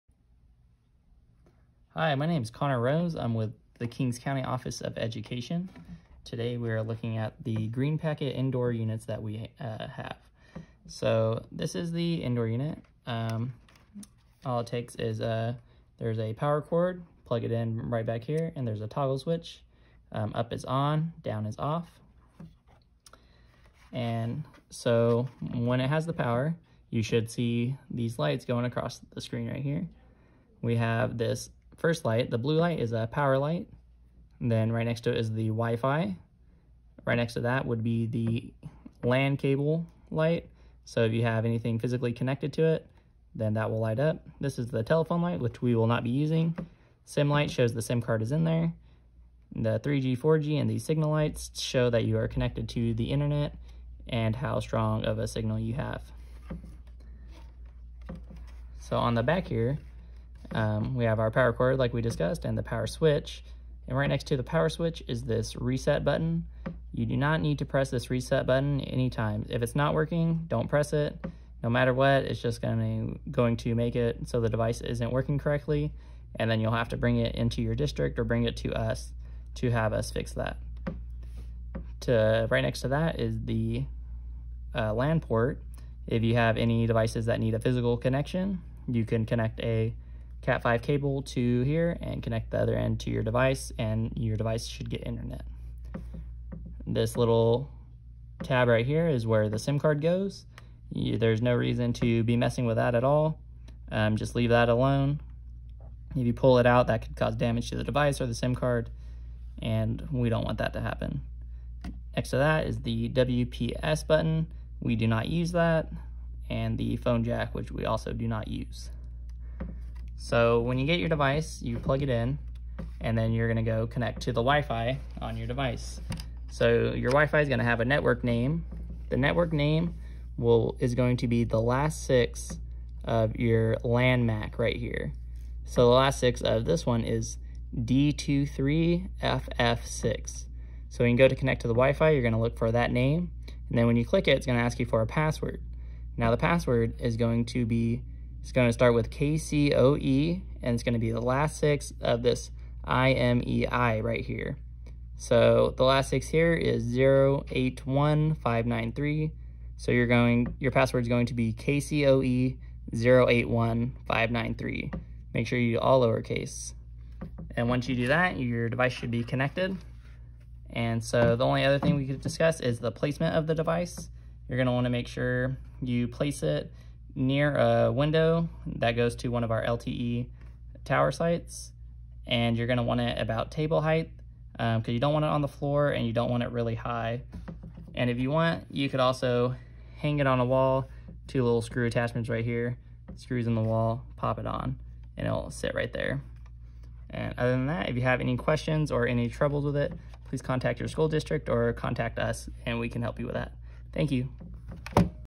KingsNet GreenPacket Indoor Unit
Basic setup of KingsNet's GreenPacket 4G LTE Indoor Unit